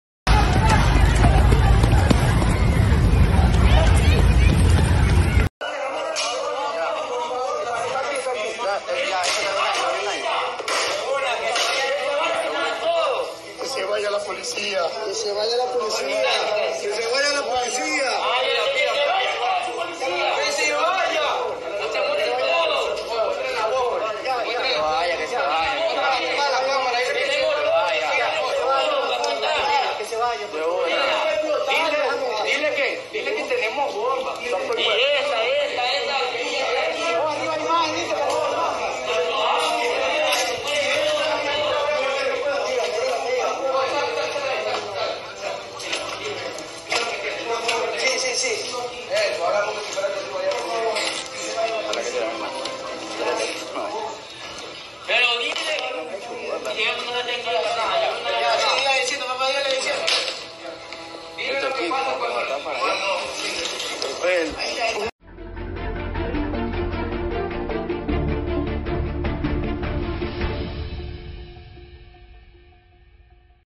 Grupos armados ingresan a la Universidad de Guayaquil, Ecuador, e intentan secuestrar a las personas
MÁS INFORMACIÓN: VIDEO: Encapuchados y armados toman la televisora TC de Guayaquil en Ecuador